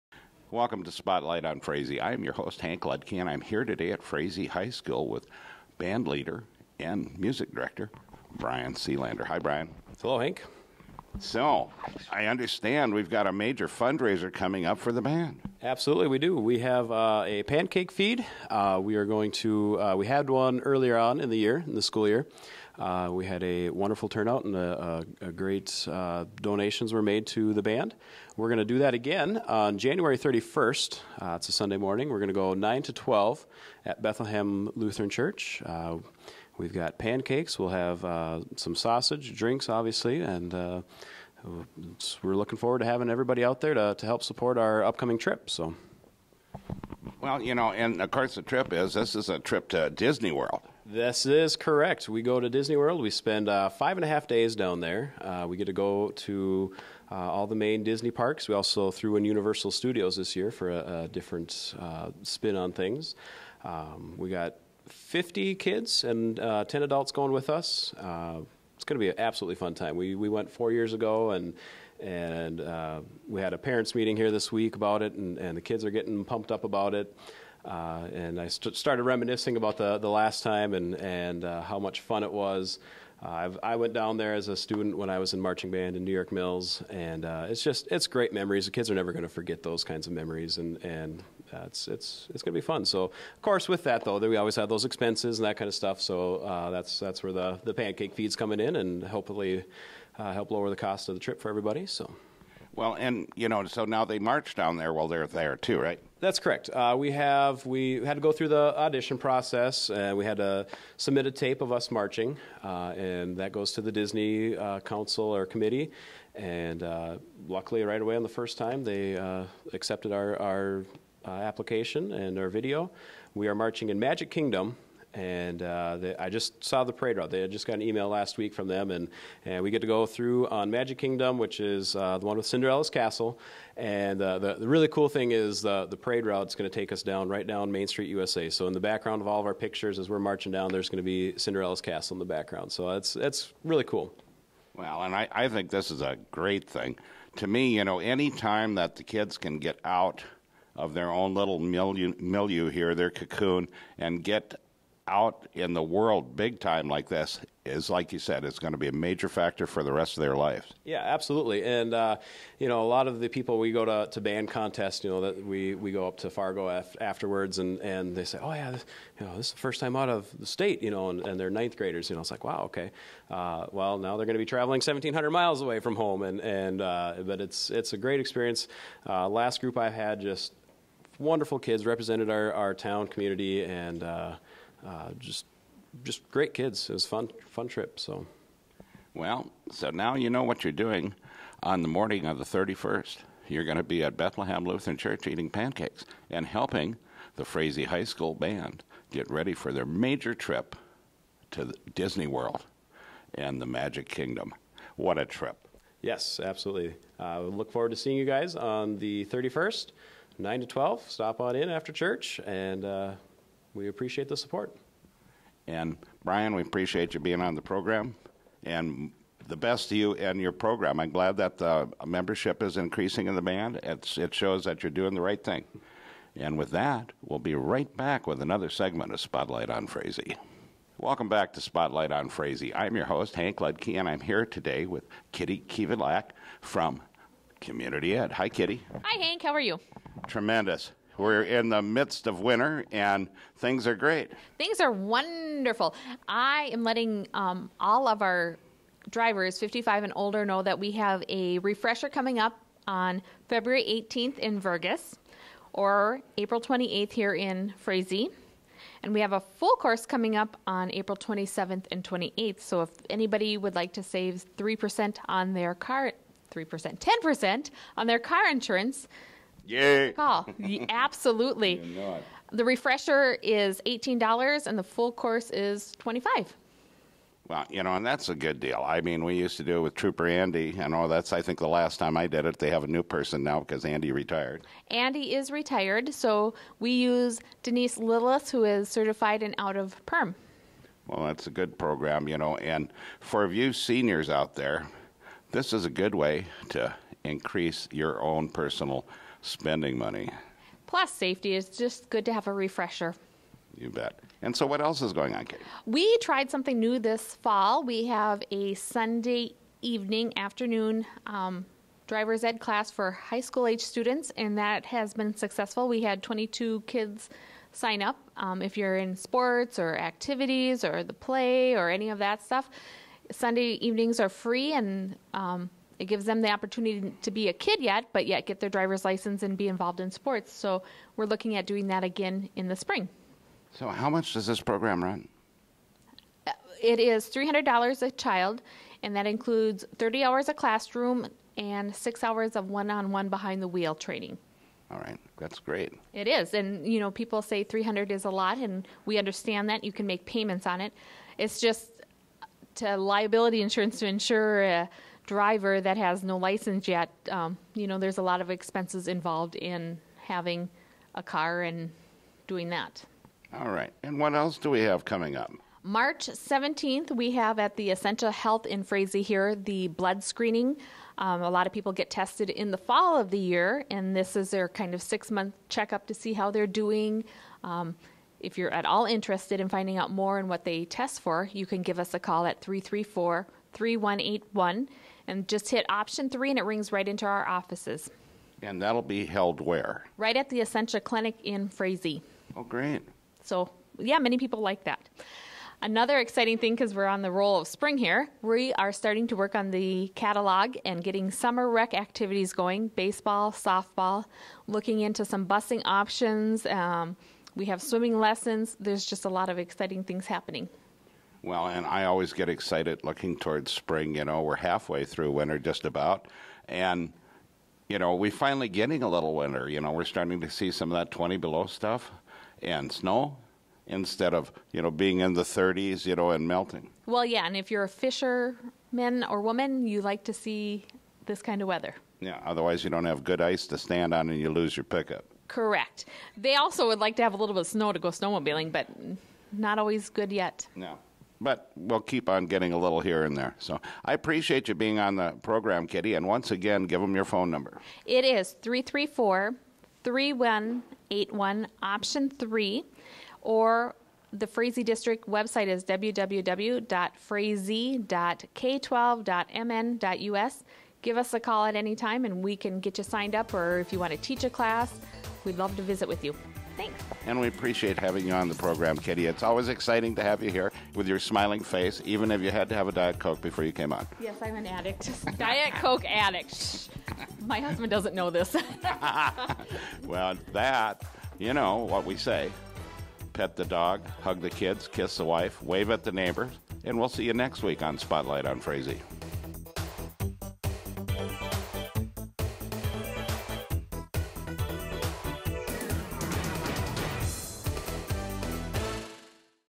Spotlight on Frazee 1 25 16
High school band fundraiser preview and Community Edu. upcoming events